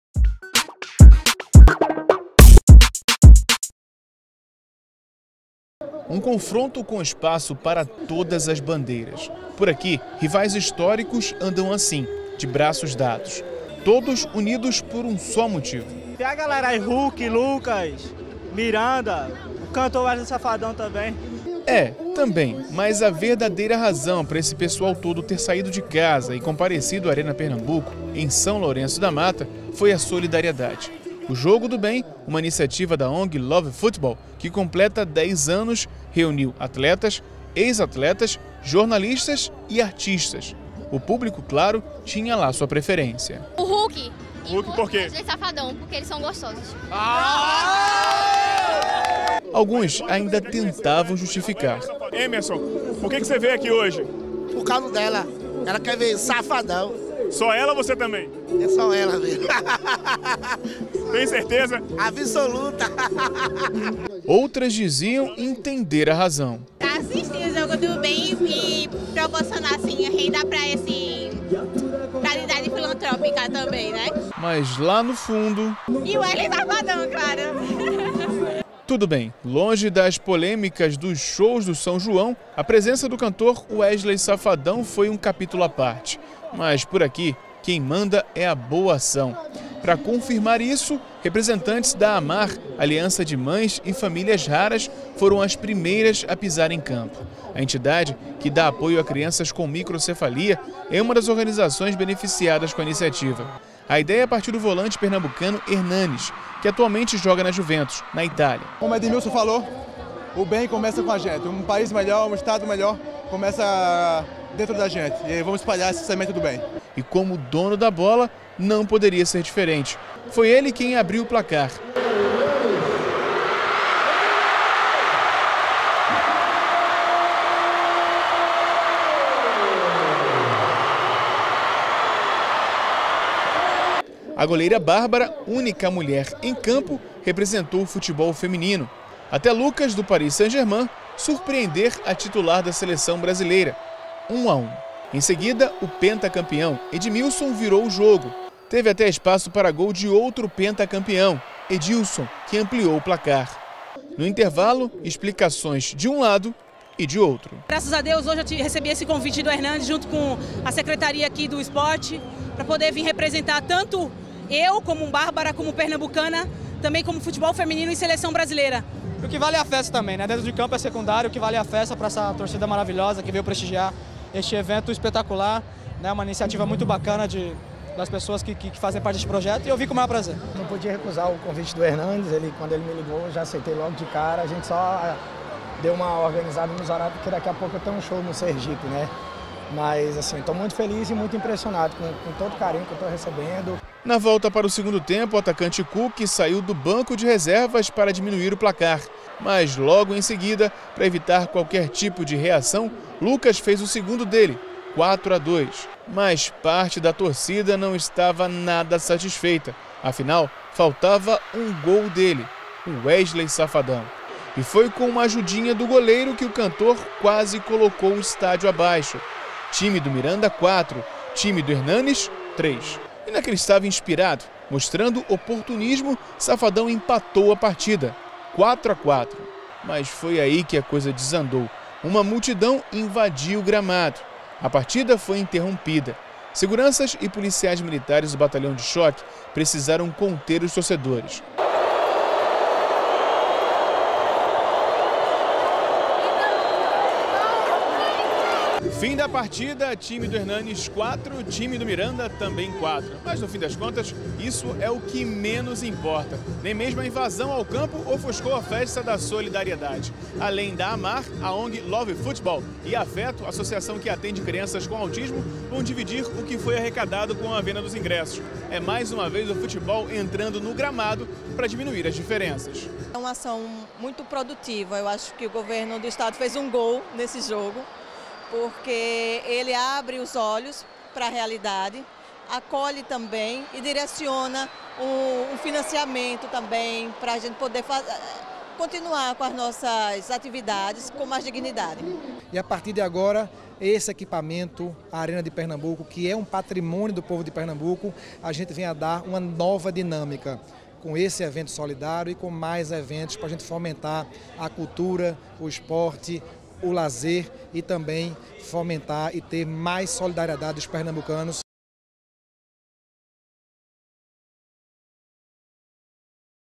[love.fútbol & Jogo do Bem] Diário de Pernambuco
Confira a matéria incrível que o Diário de Pernambuco fez para falar do Jogo do Bem, um dos nossos eventos comemorativos de 10 anos e que reuniu atletas, ex-atletas, jornalistas e artistas em prol da bandeira da solidariedade.

Mira el increíble artículo que hizo el Diario de Pernambuco para hablar sobre el partido de beneficencia, uno de nuestros eventos conmemorativos de los 10 años que reunió a atletas, ex-atletas, periodistas y artistas a favor de la solidaridad.

Check out the incredible report produced by Diário de Pernambuco about Jogo do Bem (Good Game), one of our 10-year commemorative events that brought together athletes, journalists, and artists to support the fight for solidarity.